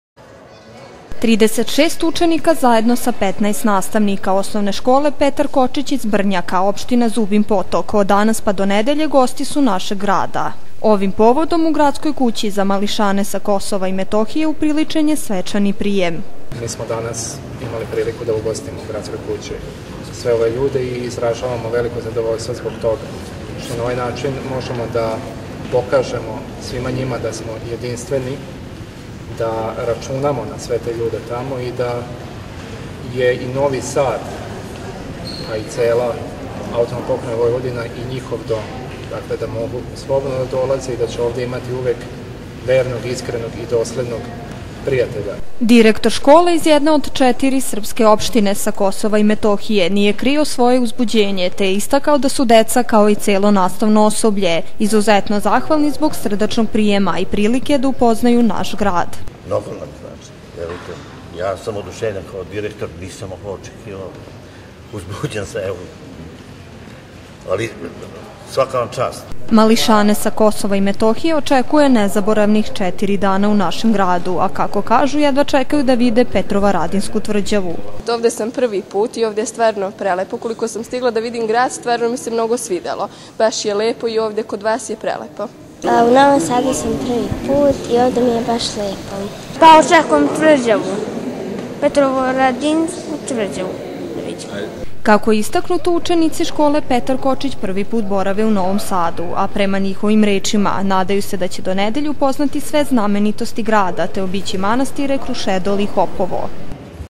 TV KANAL 9, NOVI SAD: POSETA DECE SA KOSOVA
Naš grad uvek će biti pravi domaćin svoj deci sa naše južne pokrajine Kosova i Metohije, poručeno je na pijemu učenika i nastavnog osoblja iz osnovne škole “Petar Kočić” iz Brnjaka koji se nalaze u četvorodnevnoj posti Novom Sadu.